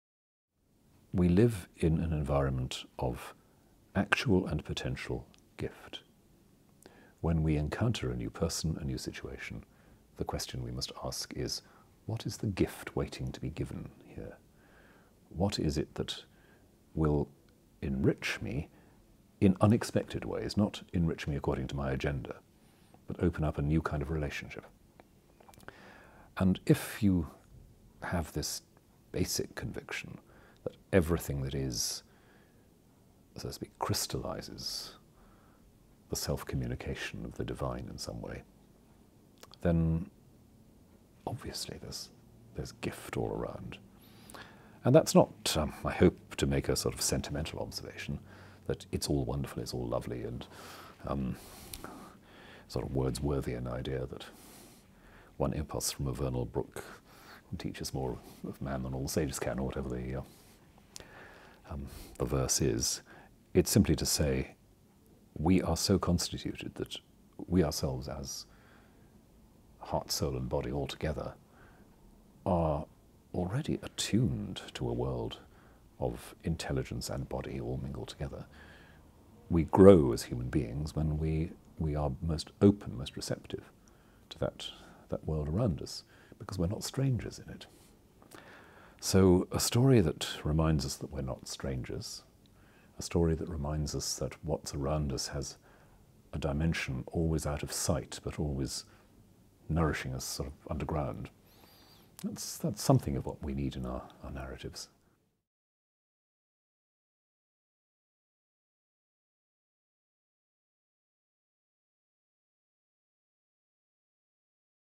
Rowan Williams: Helping others enriches us
This Christian Aid Week, we want to raise as much money as possible to help as many people who are fleeing persecution and privation as we can. But it's not all about the money! In this video clip, Dr Rowan Williams (Chair of Christian Aid and former Archbishop of Canterbury) explains that we grow and flourish as human beings when we are open and welcoming to others, to new experiences. So please give, act and pray this Christian Aid Week, 14 to 20 May 2017.